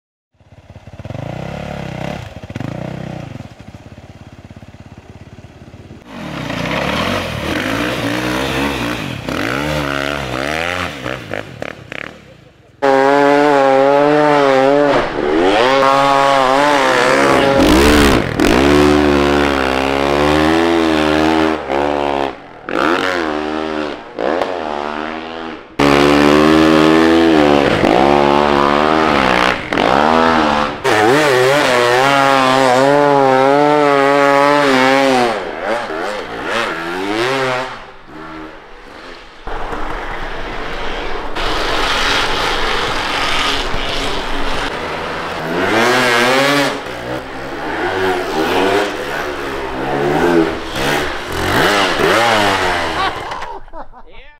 Snowbike Chilliwack, Bc
quick afternoon ride on the bikes in chilliwack